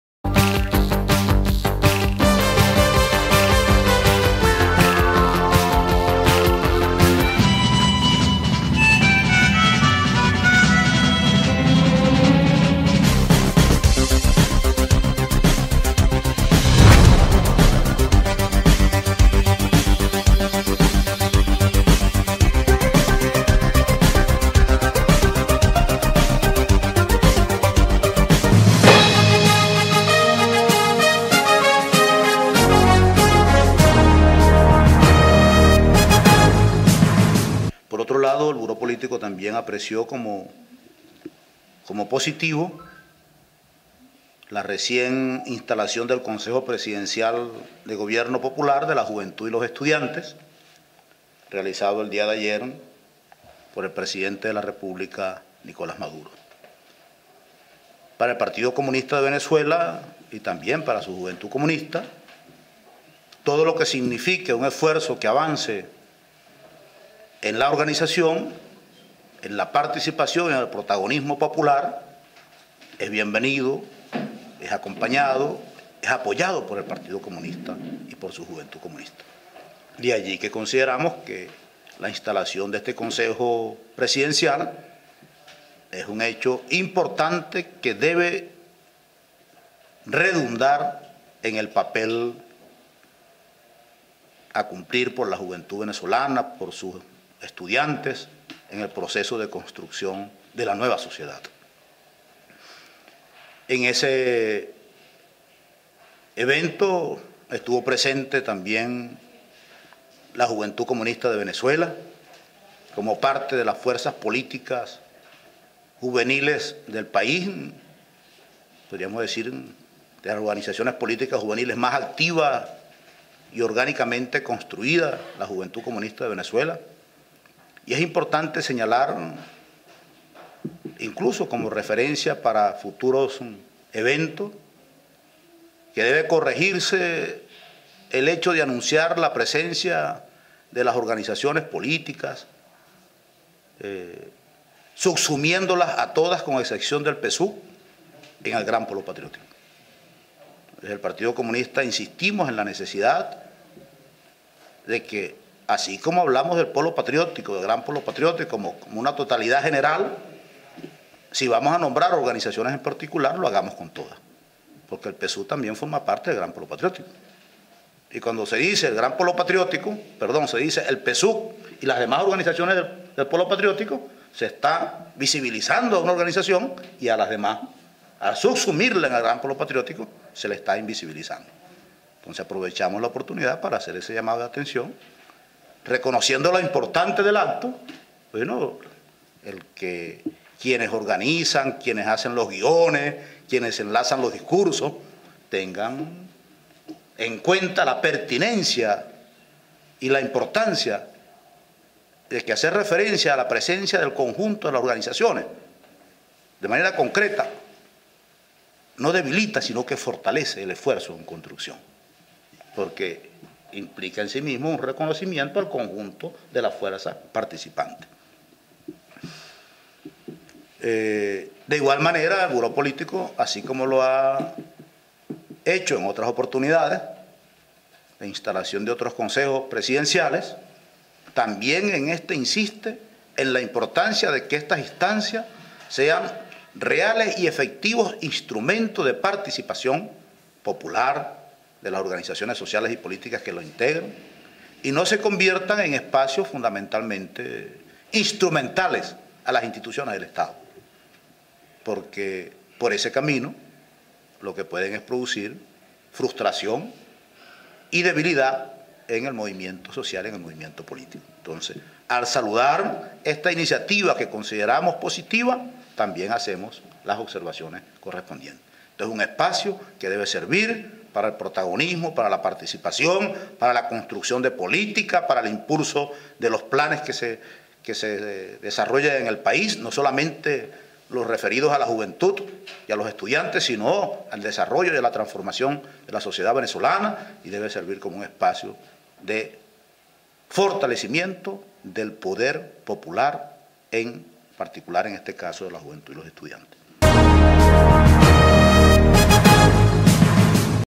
PCV: Consejo Presidencial de la Juventud no debe ser apéndices del Gobierno
Caracas, 18 may, 2015, Tribuna Popular TP.- El Partido Comunista de Venezuela (PCV) saludó hoy la instalación del Consejo Presidencial de la Juventud y los Estudiantes realizado por el Presidente Nicolás Maduro en el día de ayer, y advirtió que estas instancias no deben convertirse en apéndices del Gobierno Nacional, sino que deben marchar y desarrollarse con la independencia de las organizaciones y políticas que participan.